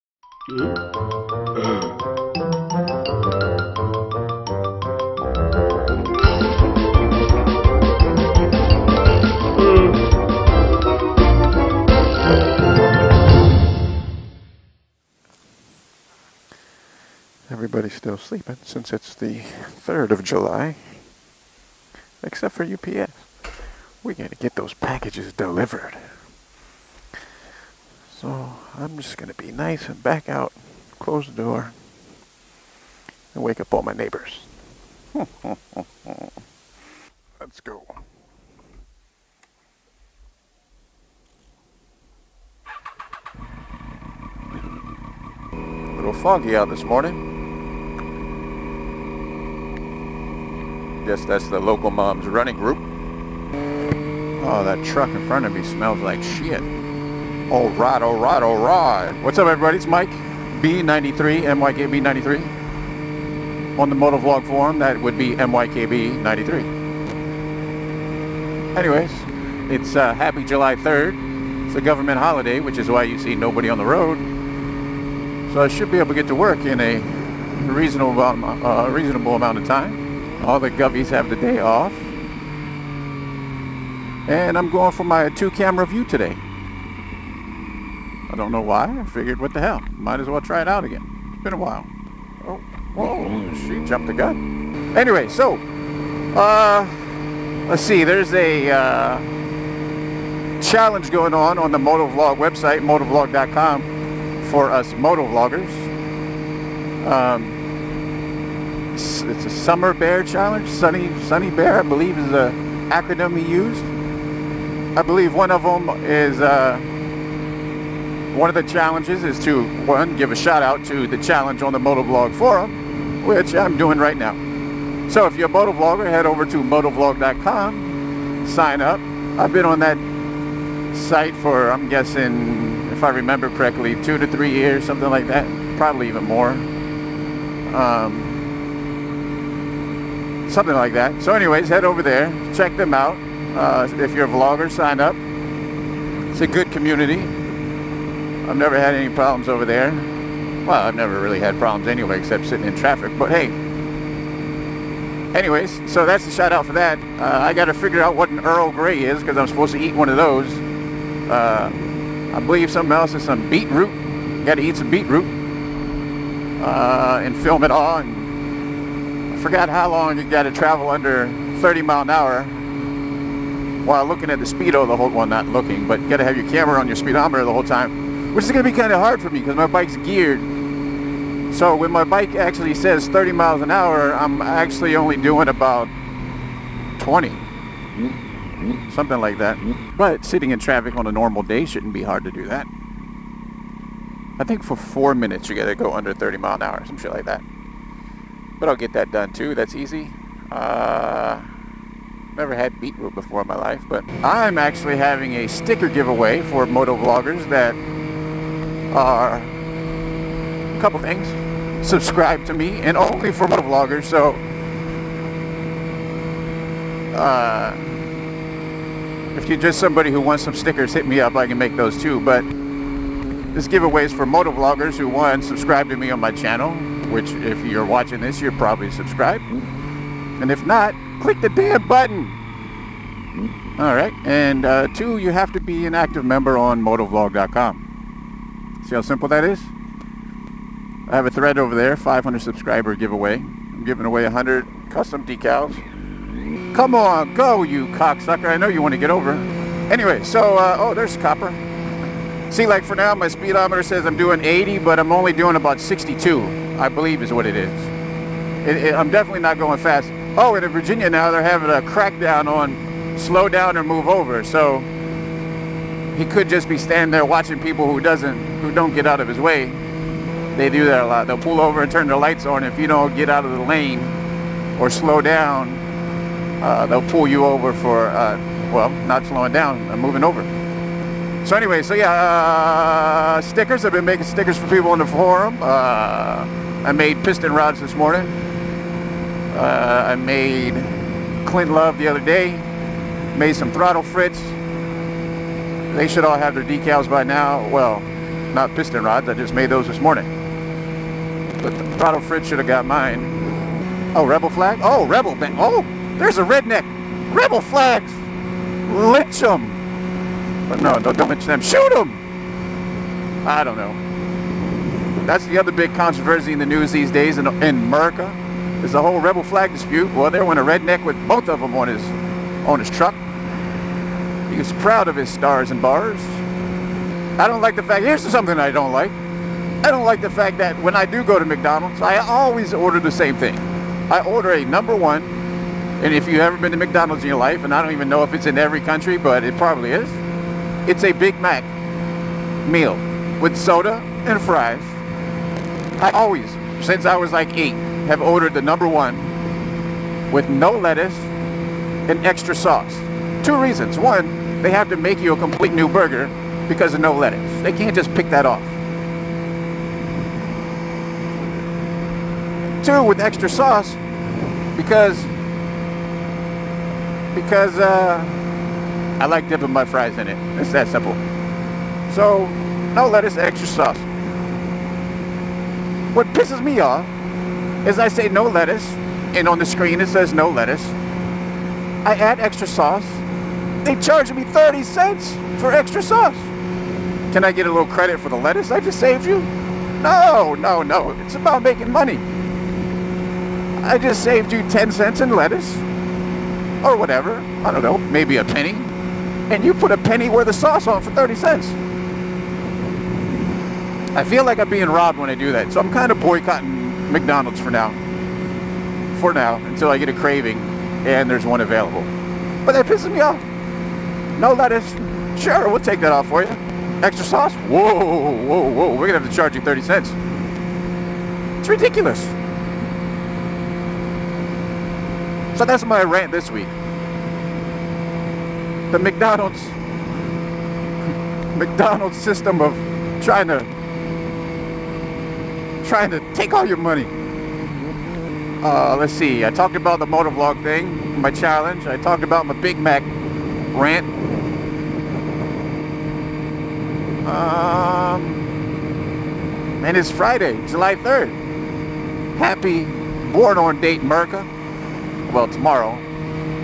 July 3rd and McD's Rant
So it has been a little since my last post but not a whole lot has been happening.

It is July 4th weekend and there was no traffic on the road for my commute to work on this morning so I just talked.

In this weeks clip I talk about the MotoVlog.com competition that is happening and the 9 things you must do in order to enter.

I ride past a redneck flying his Confederate Flags proudly on the highway and I go on a rant about McDonalds.

And yes, that is a vanity mirror and I am in it! Went for the two camera setup.